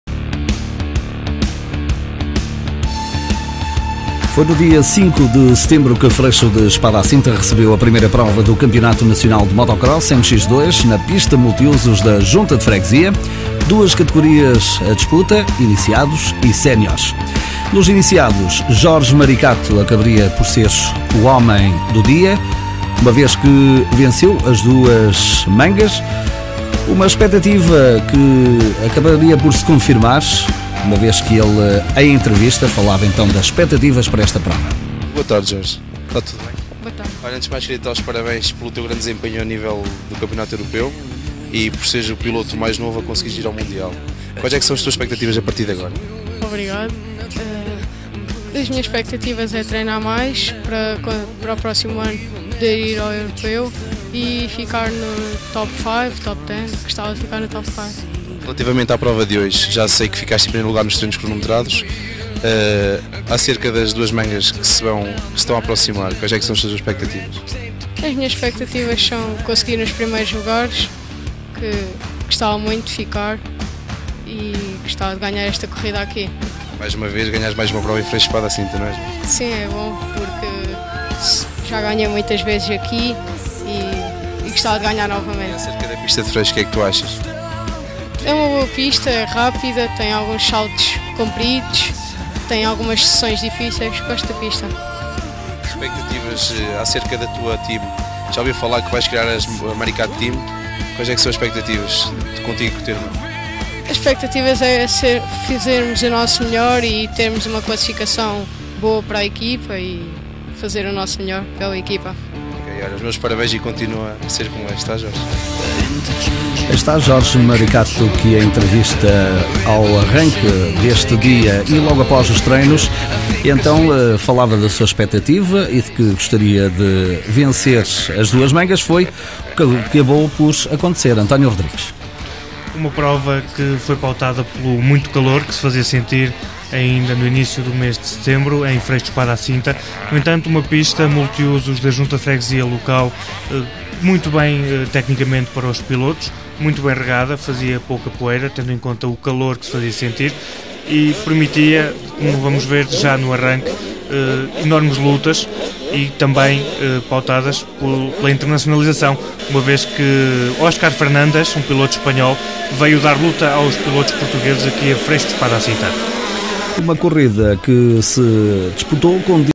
Entrevista a Jorge Maricato - Campeão Nacional de Motocross Iniciados!
Campeonato Nacional de Motocross - Freixo de Espada à Cinta

5 de Setembro 2010